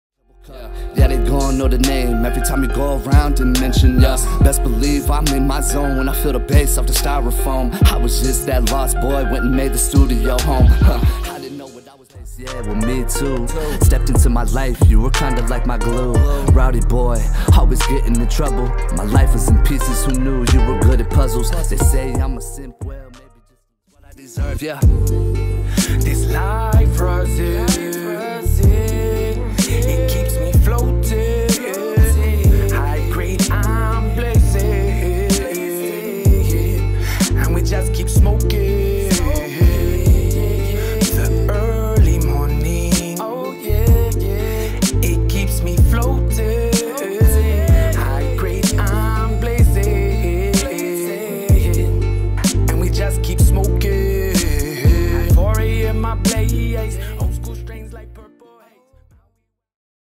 Live Performance Recap at “Strummers” in Fresno, California
Quick recap of our night at “Strummers” in Fresno, California. Shoutout to Augie & Strummers for bringing Thekid out.